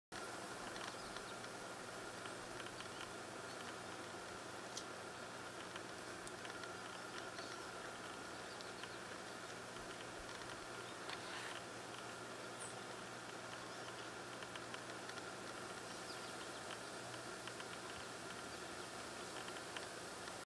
Apple Eater
One of 4 bucks that dropped by this morning to feast on our apple tree.